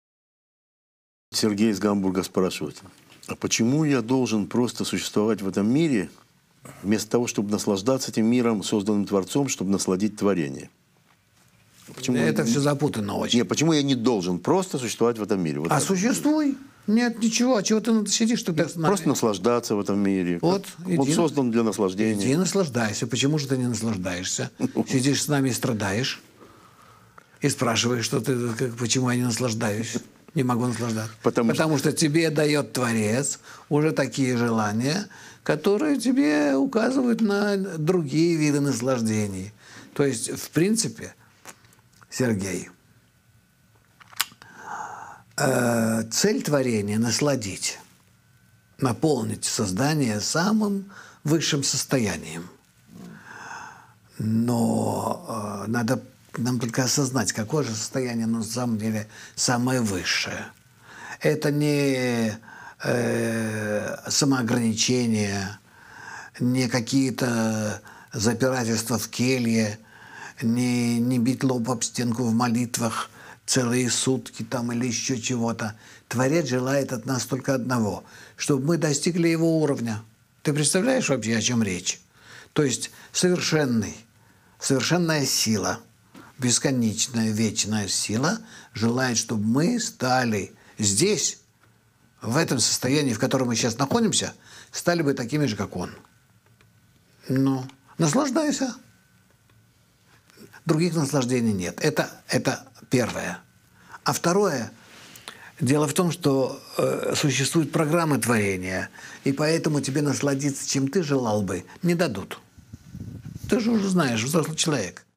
Почему мне не дают насладиться? Мудрость каббалы
Фрагмент вебинара №134
Записаться на вебинар с М. Лайтманом
Цель творения – насладить создания самым высшим состоянием, состоянием Творца. Он хочет, чтобы мы из нашего самого низшего состояния поднялись к Его уровню вечности и совершенства. Есть четкая программа осуществления Его замысла и нам не дадут, как бы нам хотелось, наслаждаться ничтожными наполнениями этого мира.